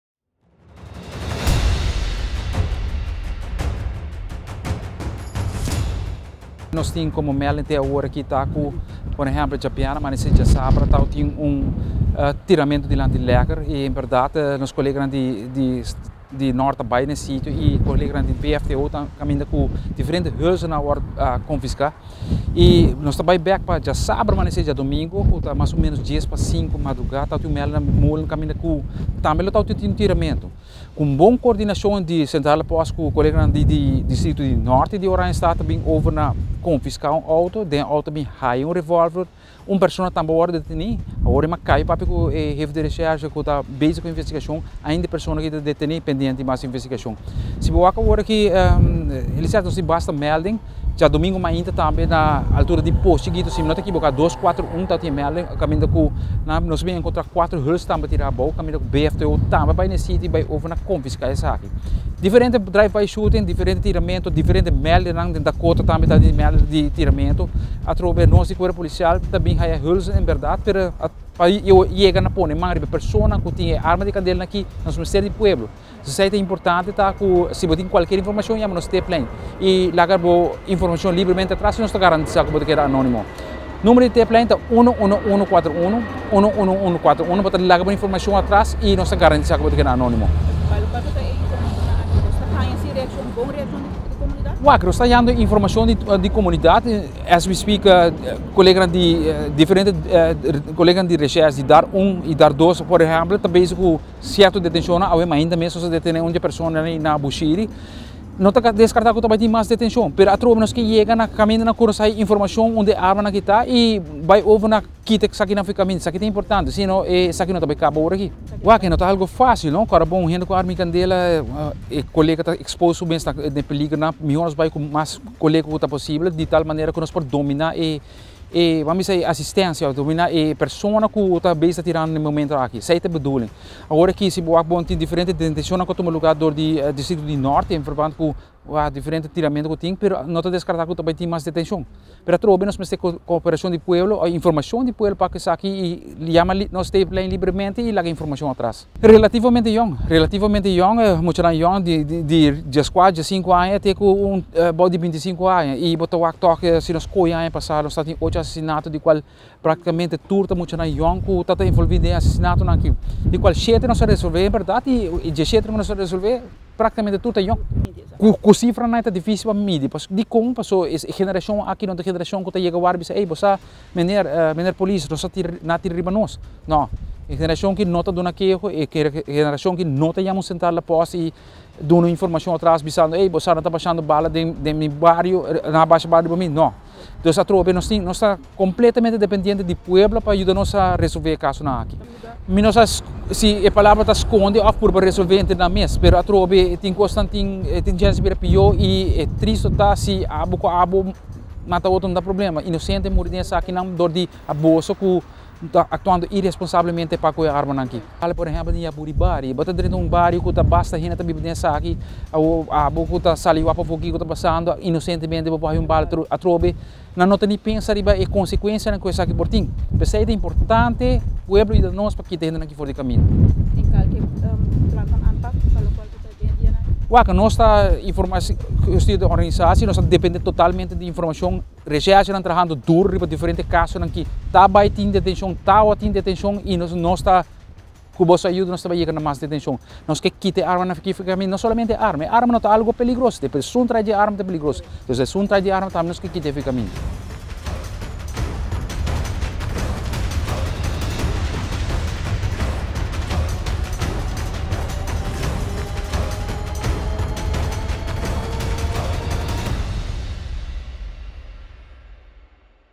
Sub-inspector eerste klasse: “E arma di candela no ta peligroso, t’e persona su tras ta e peliger”
Sub-inspector eerste klasse Lito Lacle: “E arma di candela no ta peligroso, t’e persona su tras ta e peliger”